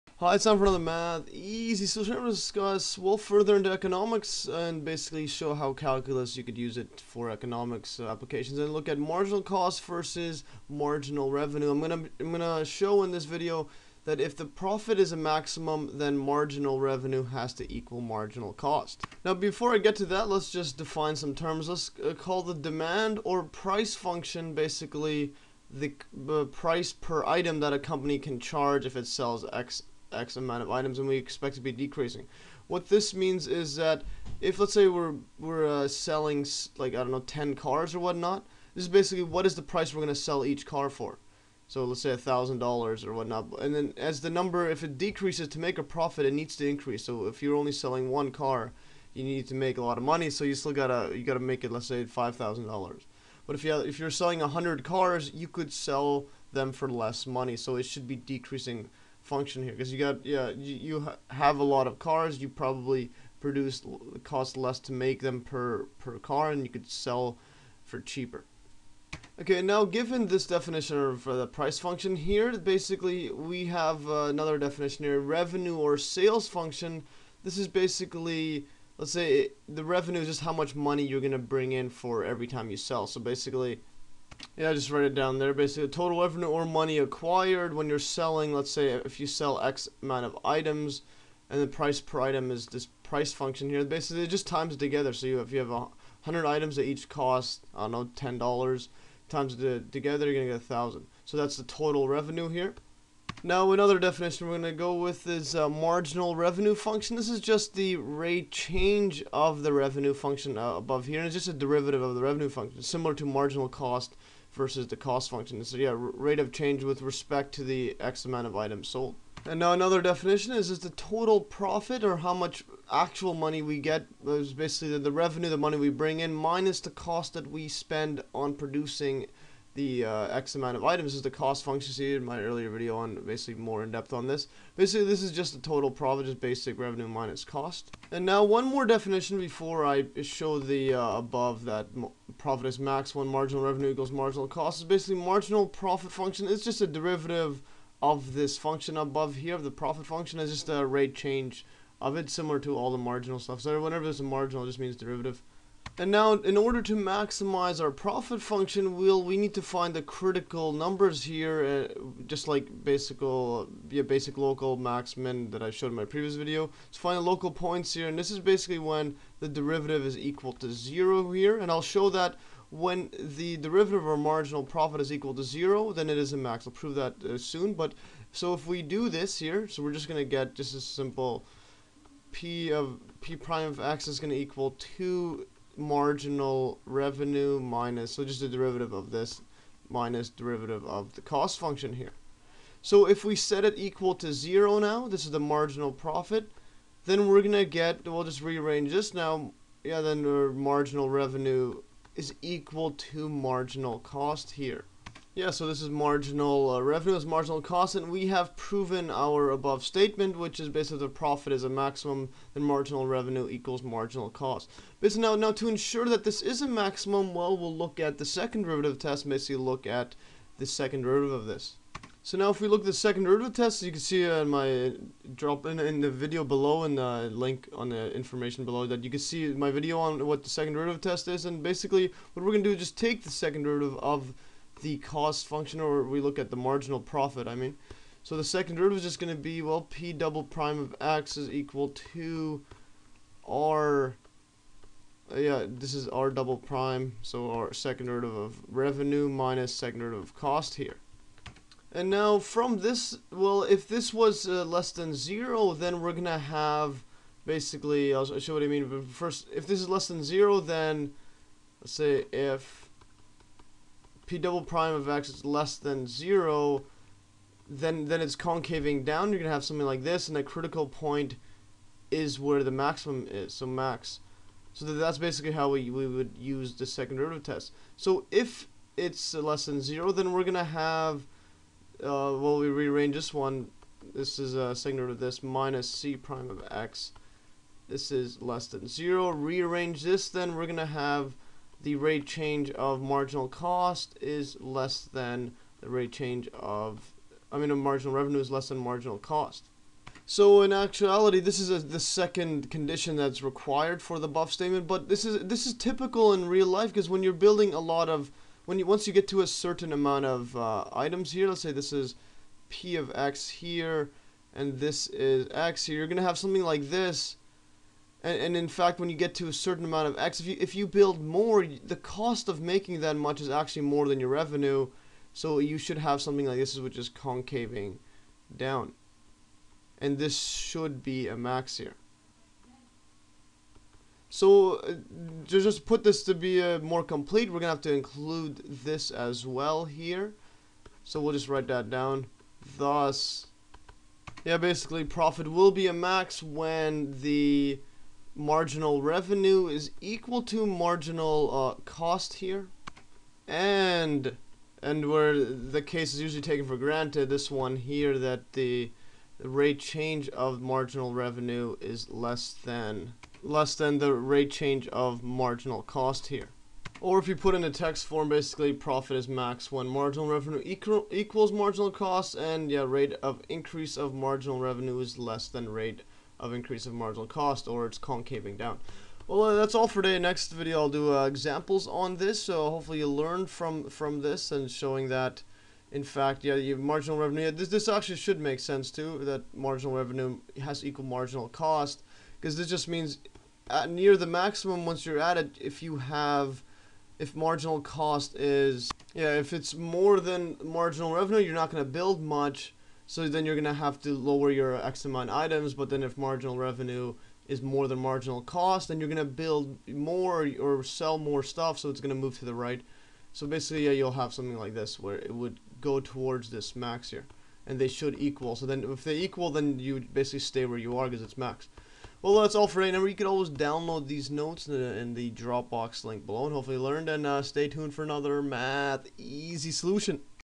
Marginal Cost vs Marginal Revenue: When is maximum profit realized?
In this video I show that when the total profit is at a maximum than the marginal revenue has to equal the marginal cost.